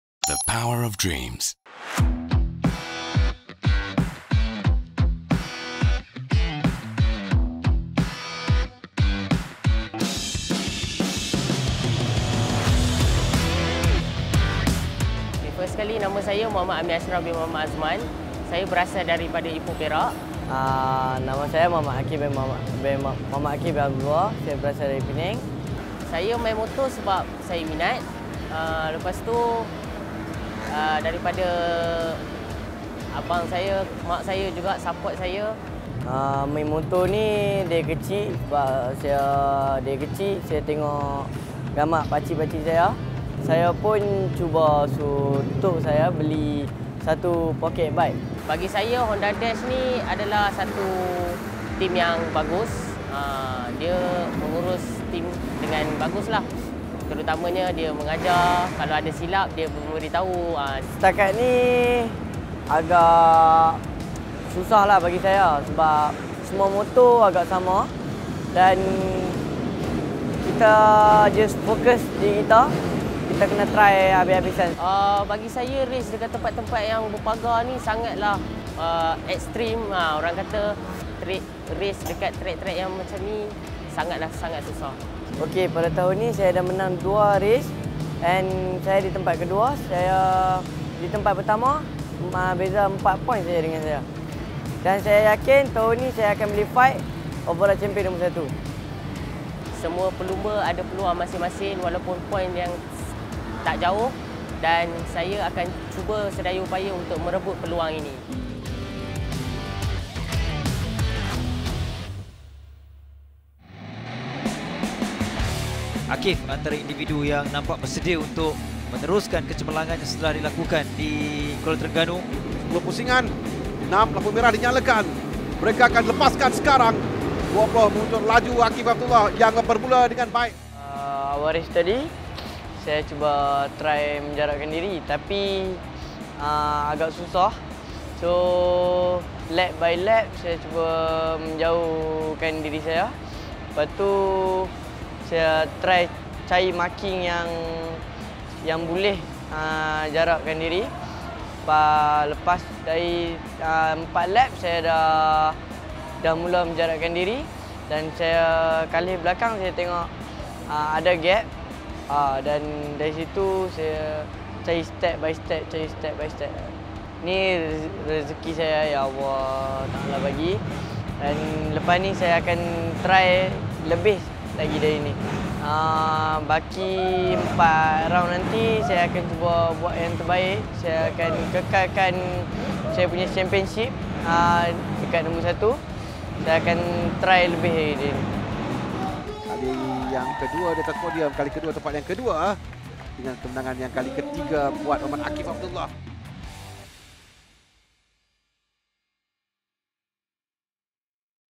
MCP EP06 | Musim 2023 : Permata Masa Hadapan
Kisah inspirasi dua bakat muda di kategori Honda Dash Challenge125! 🏍️ Jom saksikan perjalanan mereka dari minat pertama hingga ke podium di Kejohanan Cub Prix Malaysia, Pusingan 6 di Teluk Intan baru-baru ini🏆 Teruskan menyokong bakat2 Honda 💪🏻